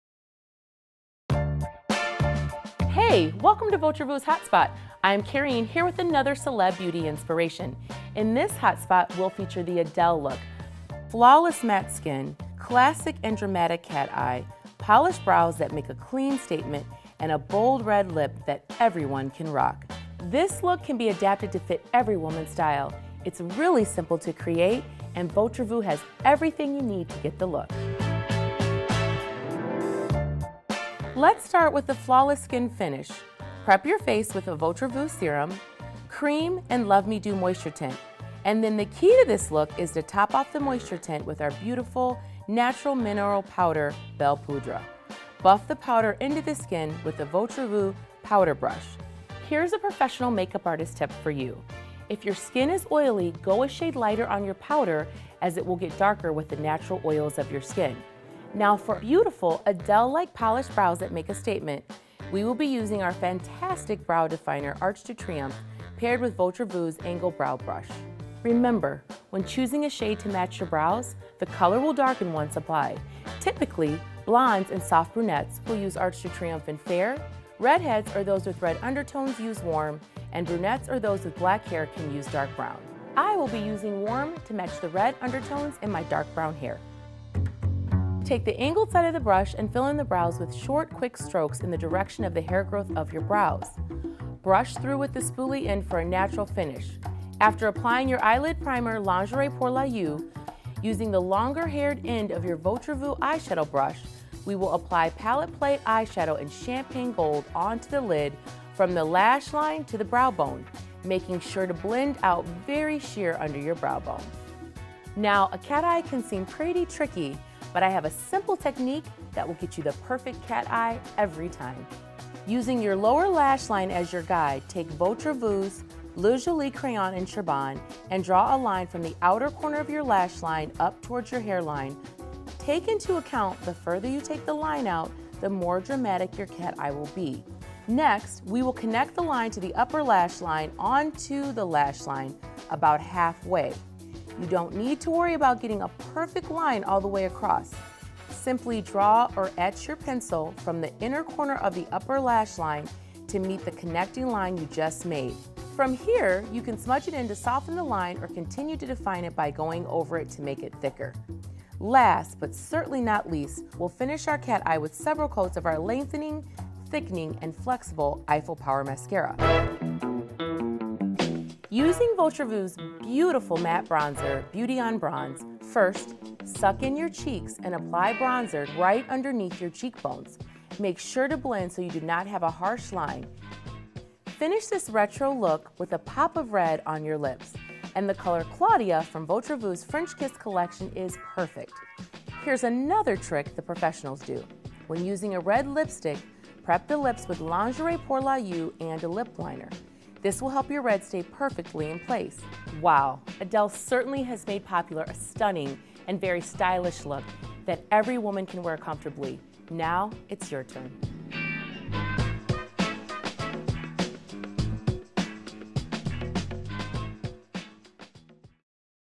Votre Vu Haute Spot: Get the Real Retro Look of Adele
From her vintage cat-eye and coiffed brows to her fully loaded lashes and perfect pout, British songbird Adele is celebrated as much for her swinging sixties style as for her powerfully sultry voice.  Watch Votre Vu's Real Retro how-to video and learn from a beauty pro how to channel this sexy songbird's rollback glam for yourself.